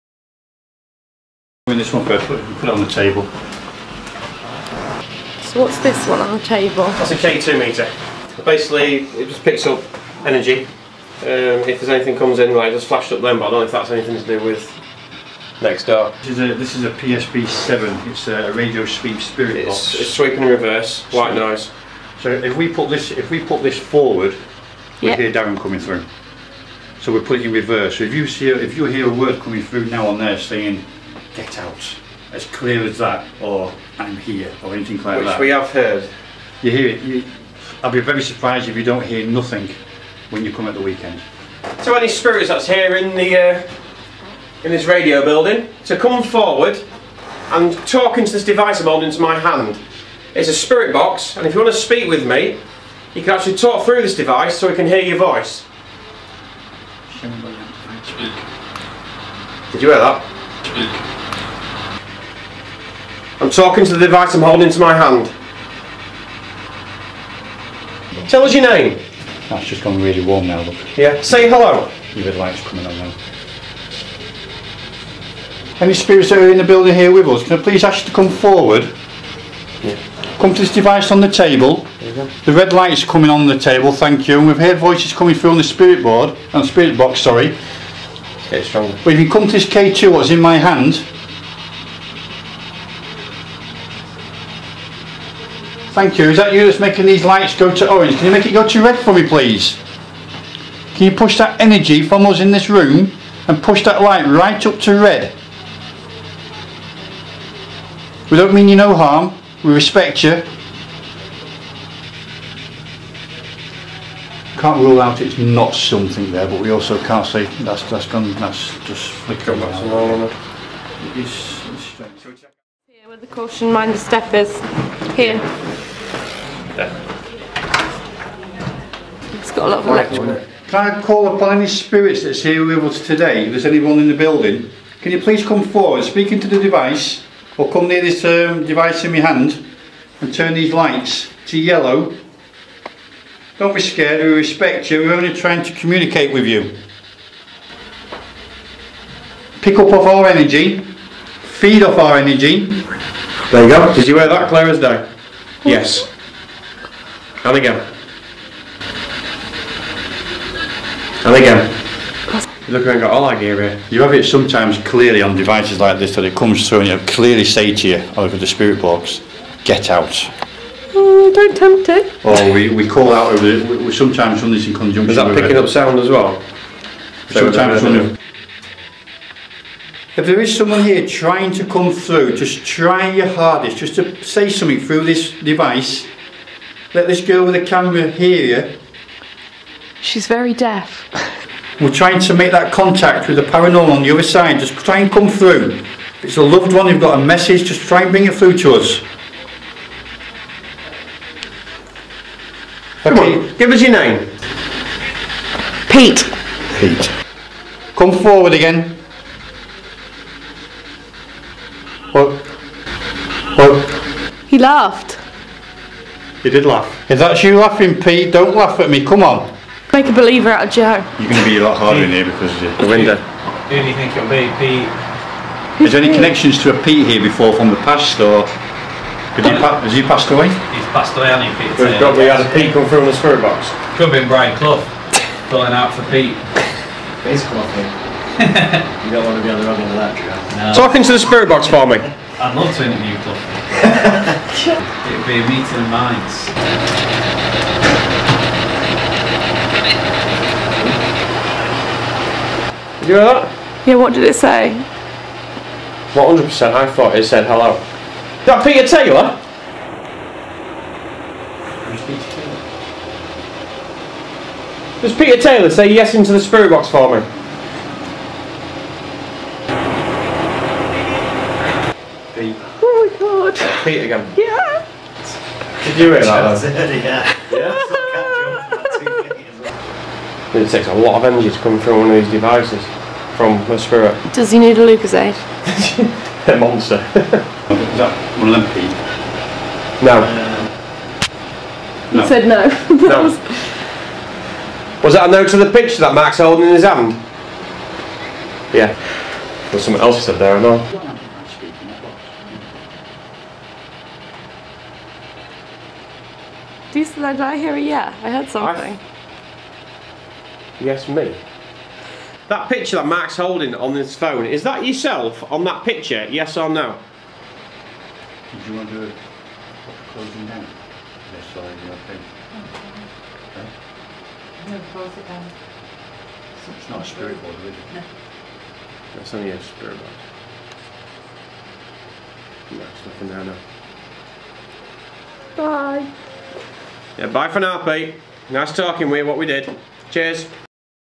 Ghost Hunting at Radio Yorkshire
Yorkshire Ghost Hunters joined Darren Harper in the studio and treated us to an impromptu ghost hunt.